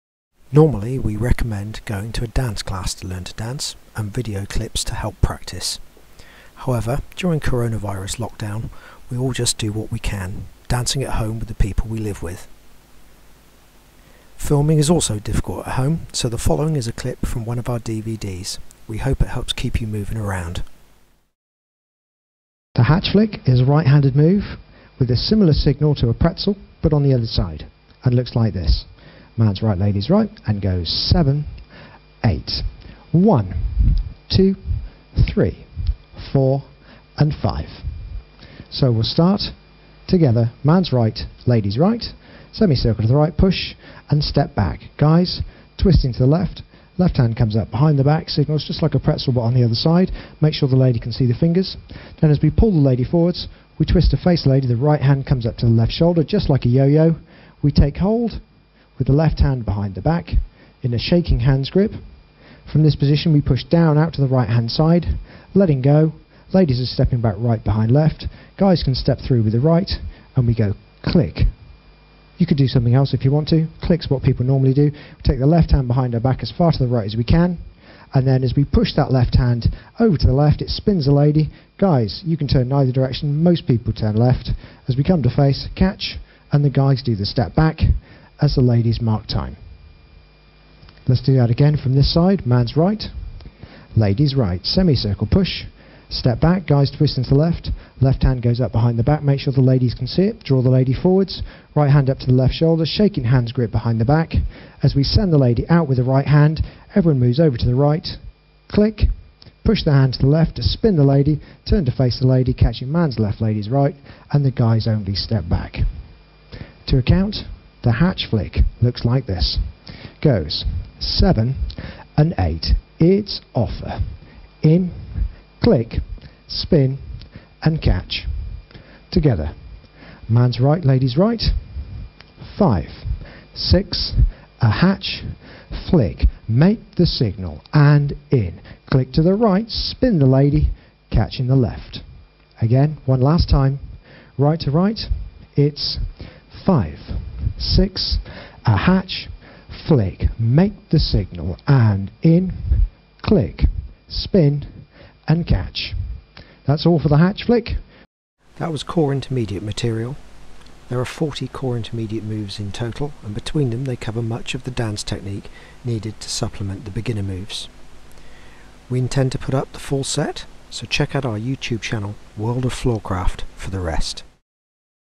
Core Intermediate - Hatchflick
Modern Jive clip showing how to do the Core Intermediate move called The Hatchflick. Put together during coronavirus lockdown to help keep the dancers entertained whilst stuck at home. Due to difficulties filming at home the instruction is from an earlier dvd release.